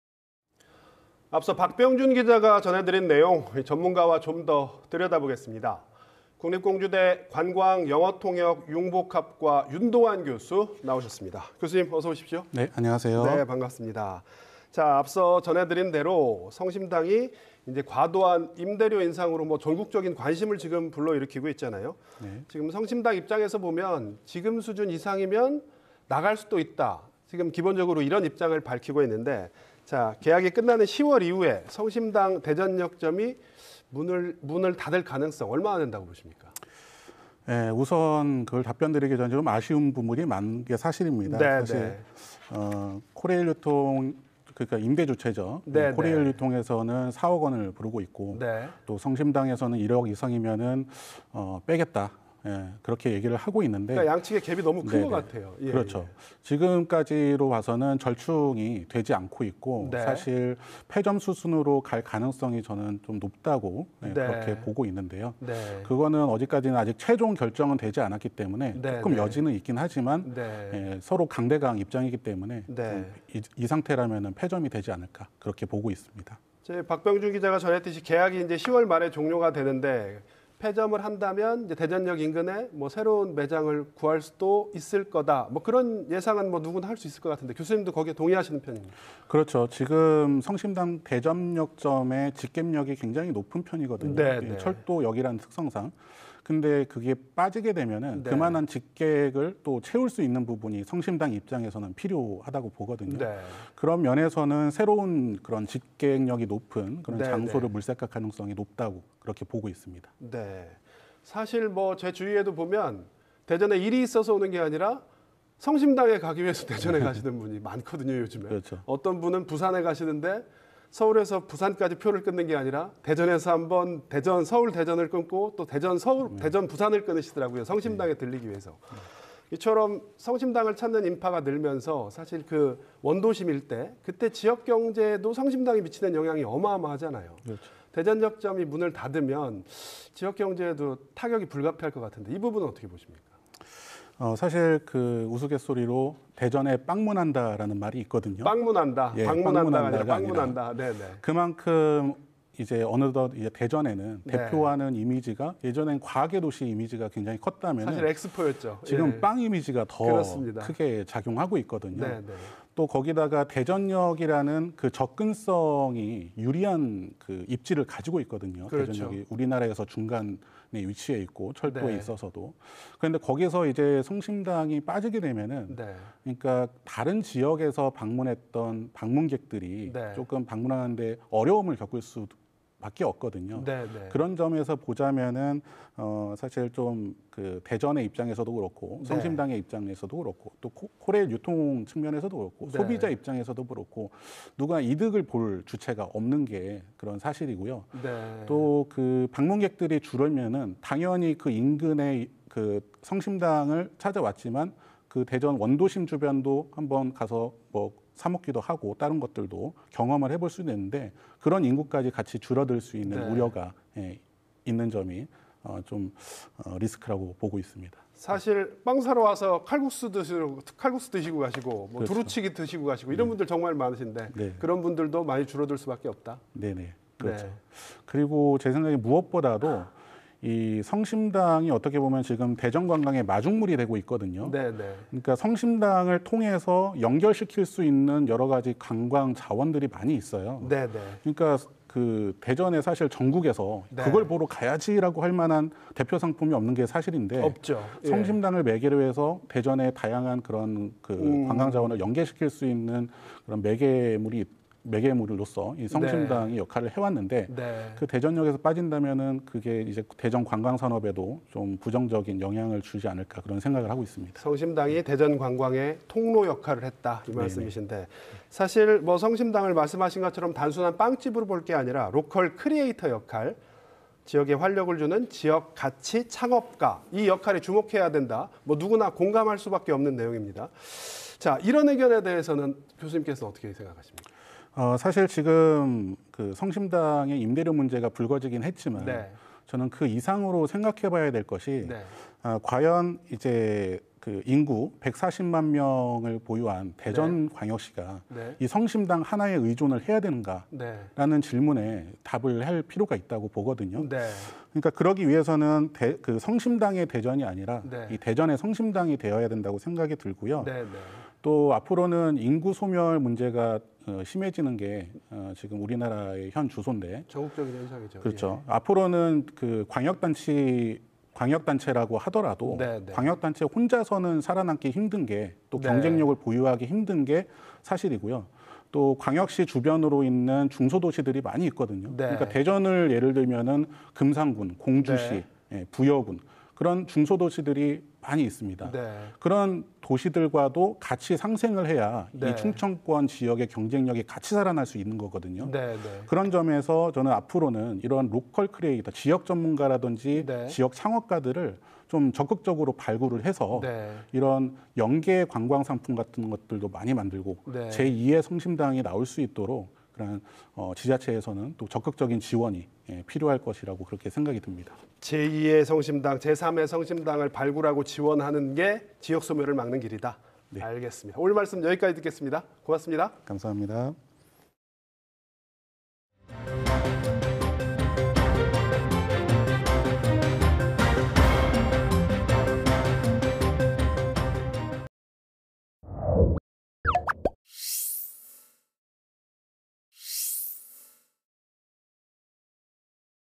"월세 1억 넘으면 나간다"…성심당, 대전역 떠날 결심? / KBS 뉴스7 대전 세종 충남 [집중인터뷰] - 05월 29일 (수)
[앵커]

 앞서 전해드린 내용, 전문가와 좀 더 들여다보겠습니다.

 국립공주대 관광·영어통역 융복합학과 윤동환 교수 나오셨습니다.

 앞서 전해드렸듯이 과도한 월 임대료 인상으로 전국적인 관심을 모으고 있는 성심당이 처음으로 입장을 내놨습니다.

 성심당 측은 월세가 1억 원 이상이면 대전역점에서 영업하지 않겠다는 건데, 폐점이 임박했다, 이렇게 봐야 할까요?

 [앵커]

 성심당 자체가 이미 대전의 상징이기도 하고, 대전역점을 방문하기 위해 대전을 찾는 사람들도 많아서, 만약 폐점이 현실화한다면 지역경제에 미칠 영향이 클 것 같거든요.

 어떻게 보십니까?

 [앵커]

 성심당처럼 지역에 활력을 주는 지역가치 창업가, 즉 '로컬 크리에이터'의 역할에 주목해야 한다는 이런 의견도 있습니다.

 동시에 지역의 역할, 지역의 책무도 중요해 보이는데, 어떤 의견 갖고 계십니까?